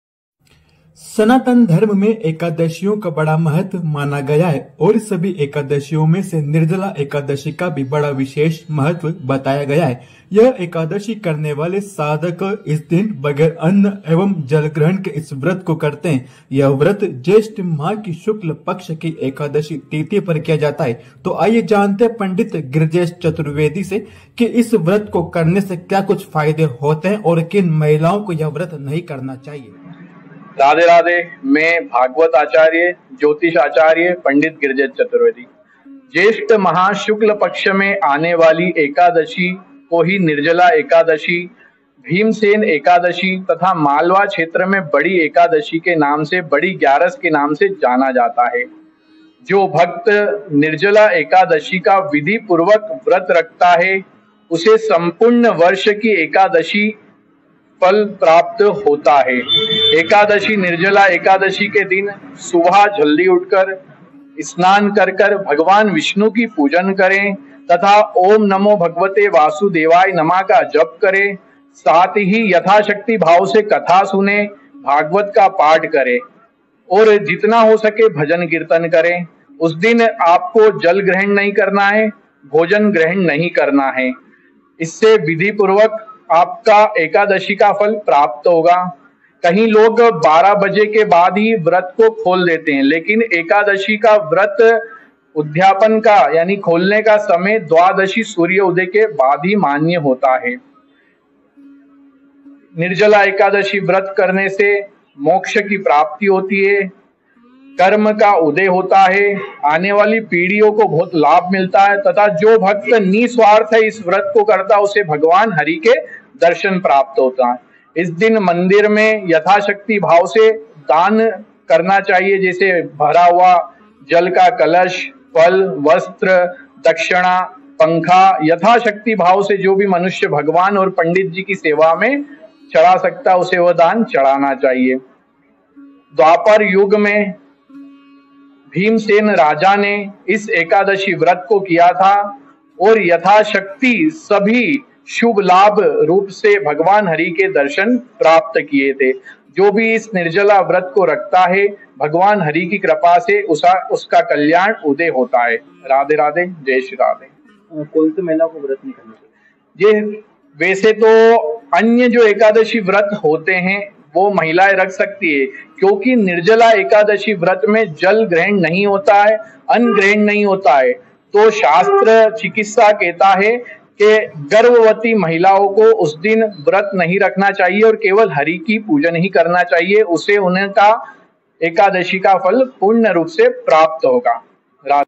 Nirjala Ekadashi 2024: निर्जला एकादशी पर क्यों करना चाहिए व्रत और जल का दान?
Nirjala Ekadashi 2024: Ekadashi was considered to be of great importance in Sanatan Dharma. Among all the Ekadashis, Nirjala Ekadashi has special significance. On this day people observe this fast without taking food and water.
Nirjala Ekadashi 2024: सनातन धर्म में एकादशी का बड़ा महत्व माना गया. सभी एकादशियों में से निर्जला एकादशी का विशेष महत्व माना गया है. इस दिन लोग बगैर अन्न और जल ग्रहण के इस व्रत को करते हैं.